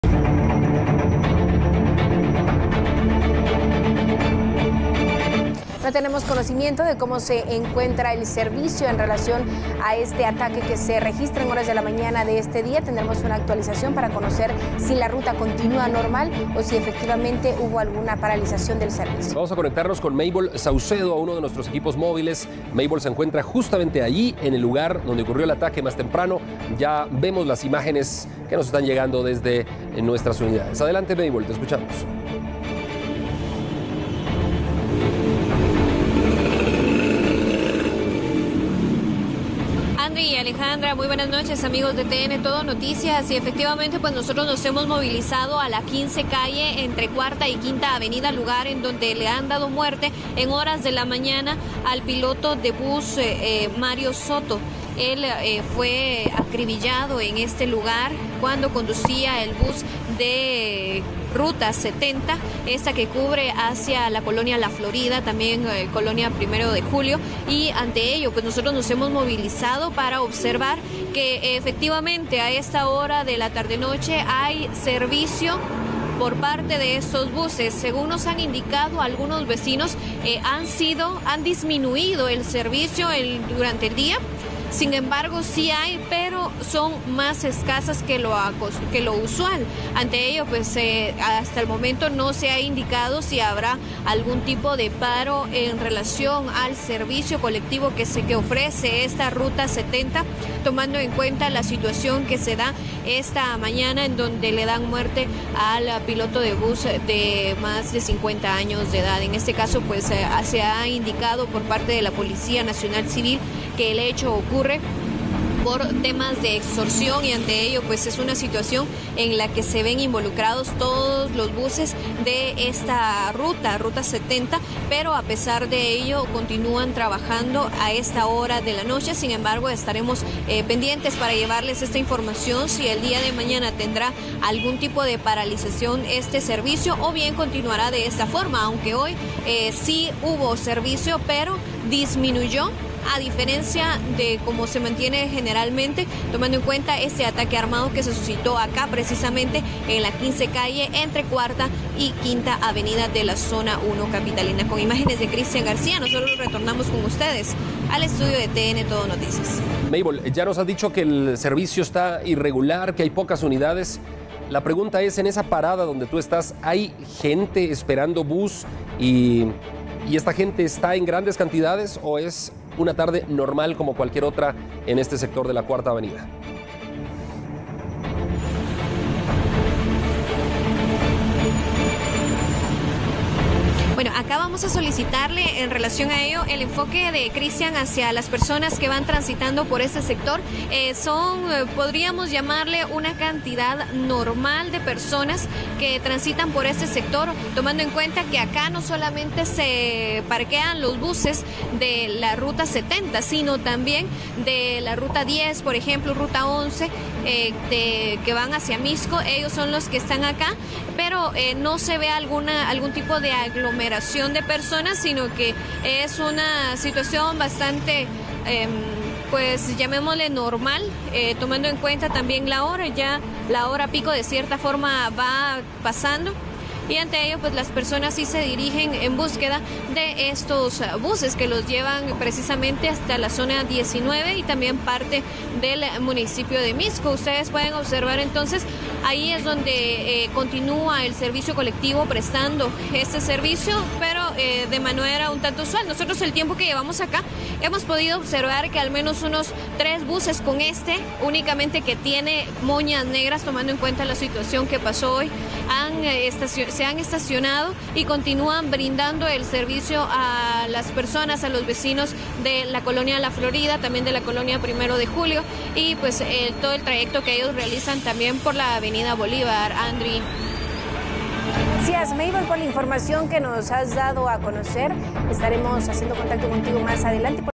Buses siguen circulando en zona 1 capitalina
Se mantiene la actividad a pesar del asesinato

Tras el ataque que se registró durante la mañana el servicio de buses en la zona 1 capitalina continúa en actividad.